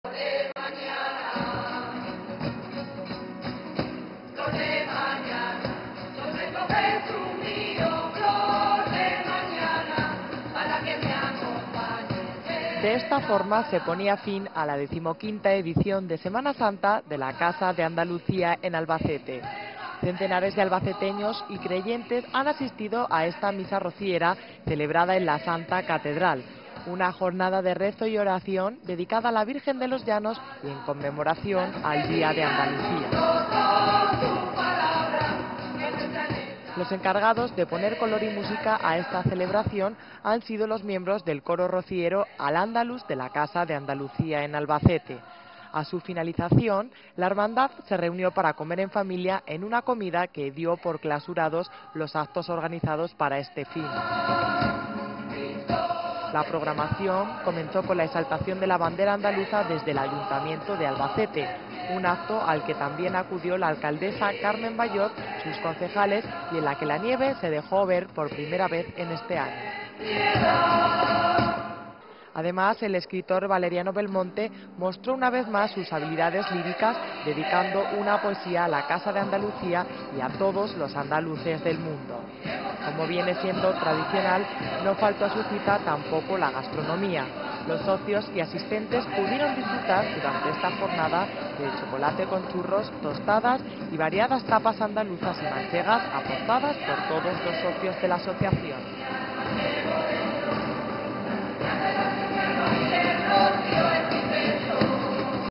Misa rociera en la catedral
El Coro Rociero Al-Ándalus ha sido el encargado de poner el broche de oro a la Semana Cultural de la Casa de Andalucía en Albacete